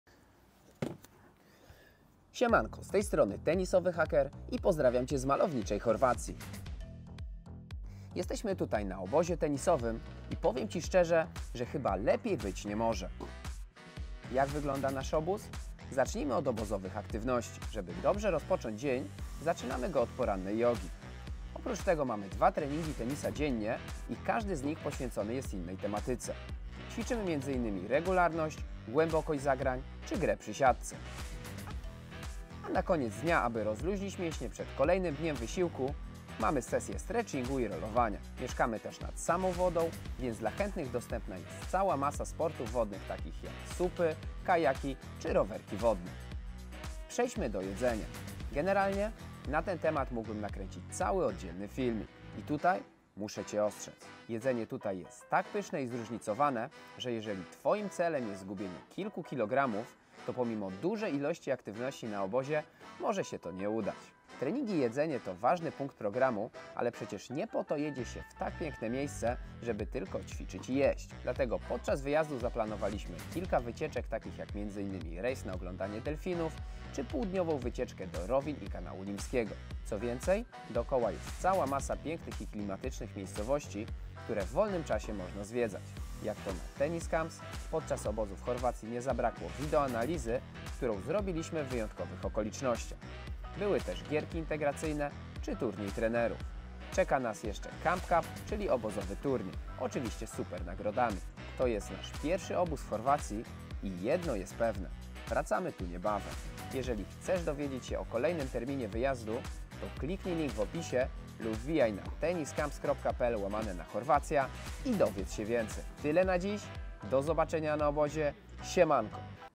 OBÓZ TENISOWY W CHORWACJI
Chorwacja jest to jedno z najlepszych miejsc, gdzie do tej pory zorganizowaliśmy obóz tenisowy.
Z filmiku dowiesz się jak zorganizowany jest taki obóz.